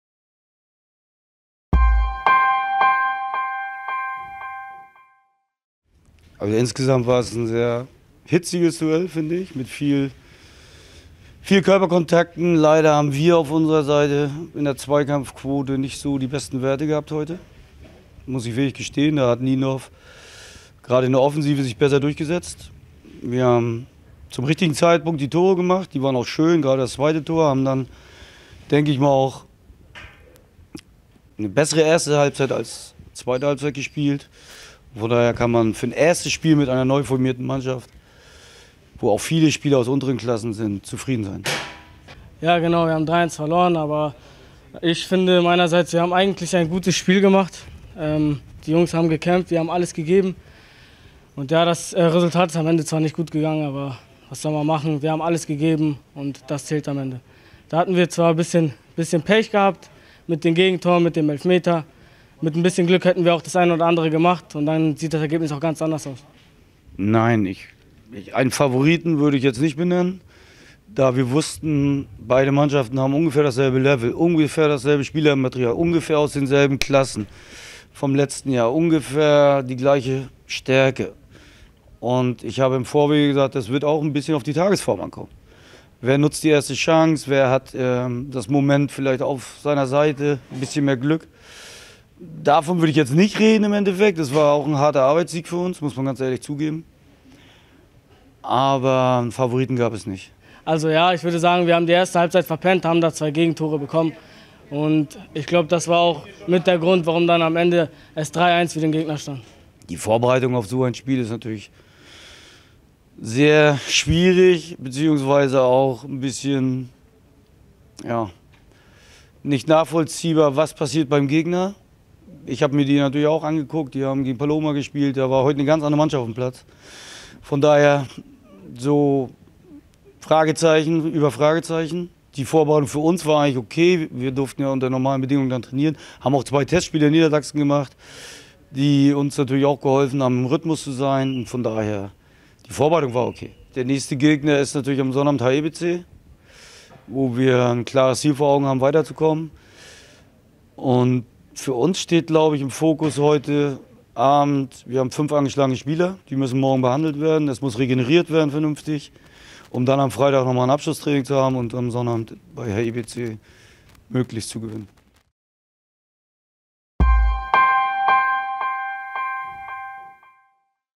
FC Eintracht Norderstedt U19 - Niendorfer TSV U19 (Achtelfinale, Pokal)
Norderstedt zieht ins Viertelfinale ein

Achtelfinale (12.08.20)

Endergebnis: 3:1

Tore:
1:0 Tom Burmeister
2:0 Ede Zimmermann
2:1 Mo Abdel Aal Ali
3:1 Benjamin Dreca